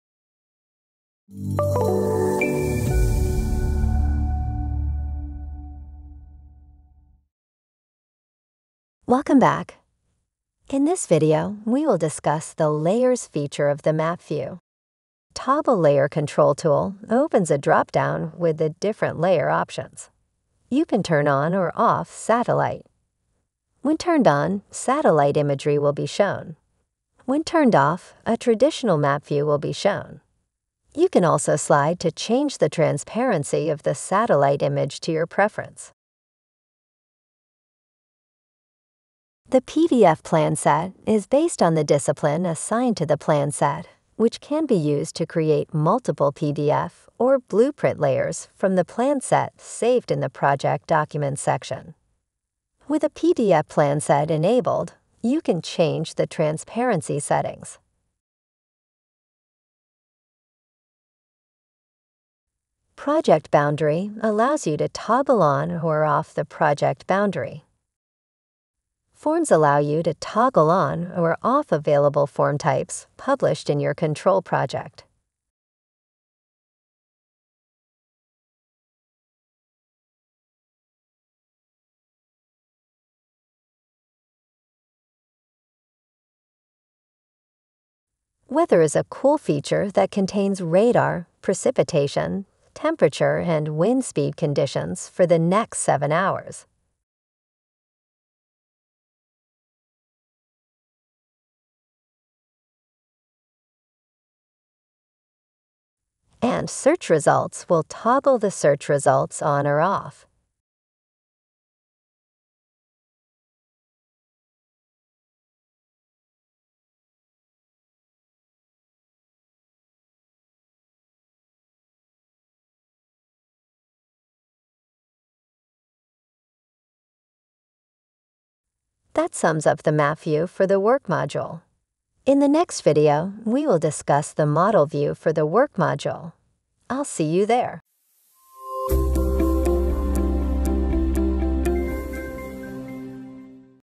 SYNCHRO Control: Work - Map View Layers
Learn how to efficiently explore the diverseLayer features available within the map view of the forms.

1. Satellite
2. PDF plan set
3. Project Boundary
4. Forms by type
5. Weather data
6. Search Results